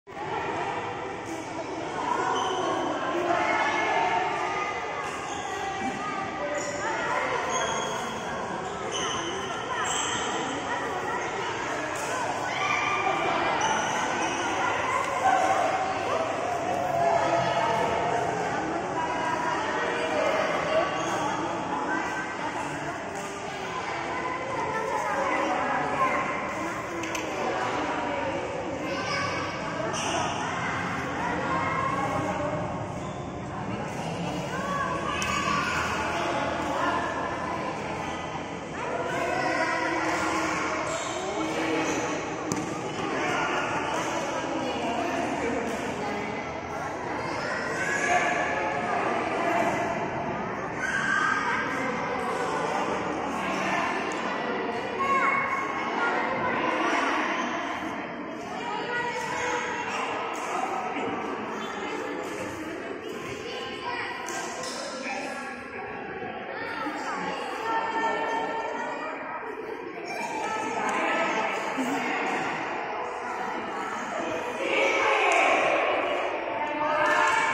Anak kecil sudah bisa menguasai jurus silat good job
anak kecil sudah bisa menguasai jurus silat dasar good job terus berlatih.